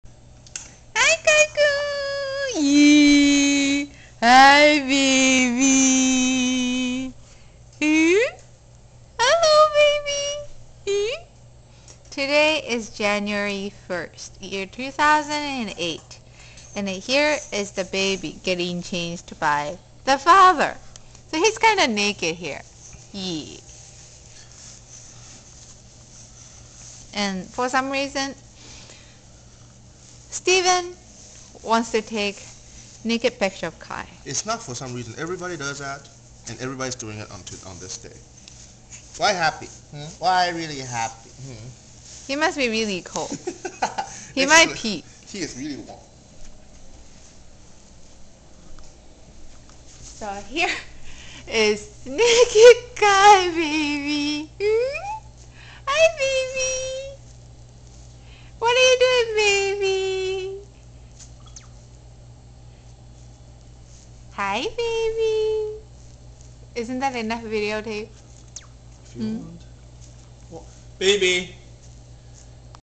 Kai's Photo Session
Kai getting his pictures taken at 3 month old.